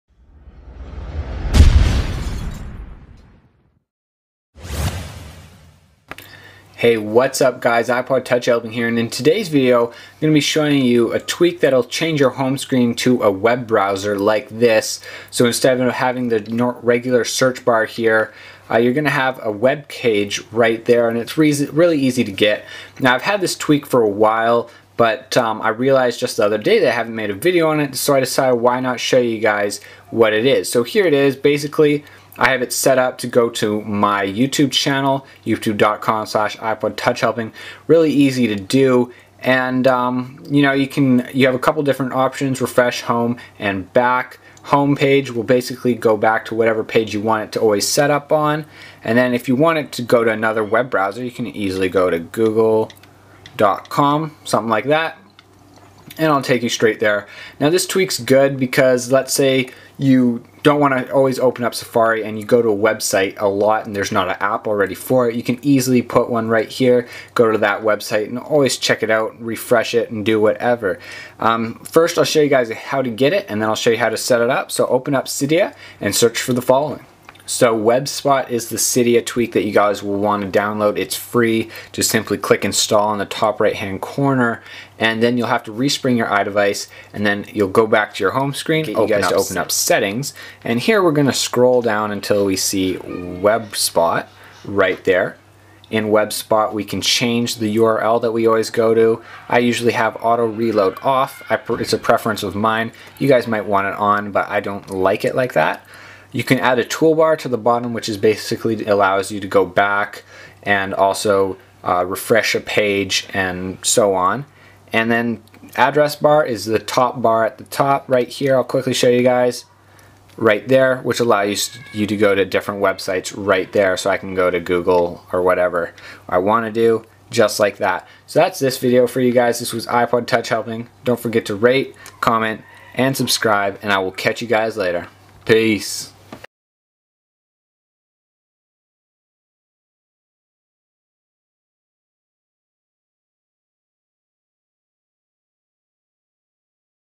Change Spotlight Search to a Web Broweser on iPhone, iPod Touch, iPad "WebSpot"
"WebSpot" is a simple cydia tweak that allows you to change your spotlight search to a web browser.